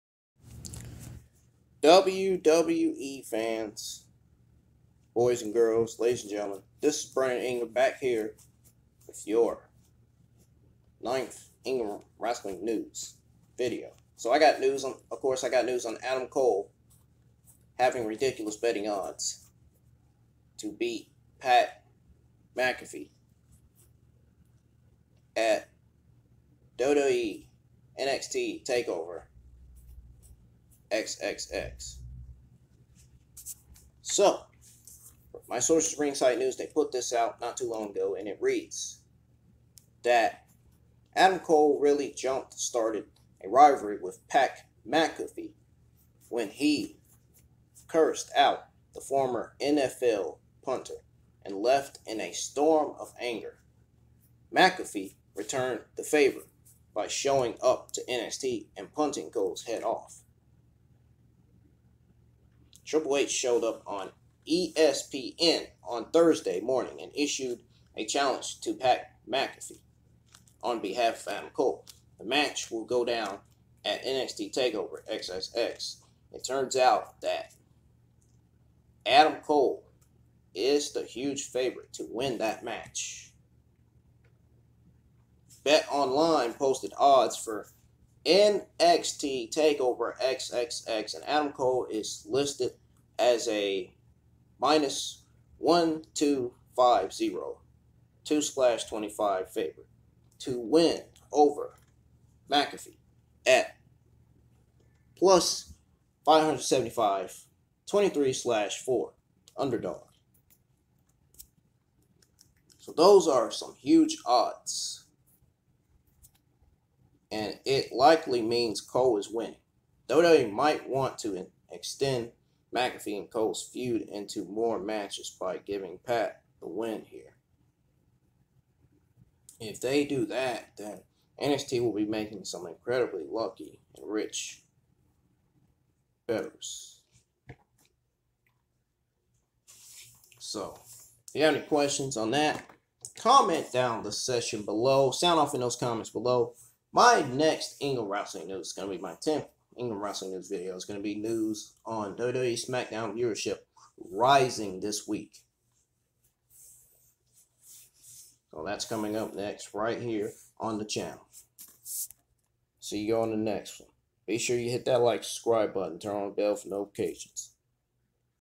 Adam Cole Has Ridiculous Betting Odds To Beat Pat MCAFEE At WWE NXT Takeover: XXX | Ingram Wrestlin
Adam Cole Has Ridiculous Betting Odds To Beat Pat MCAFEE At WWE NXT Takeover: XXX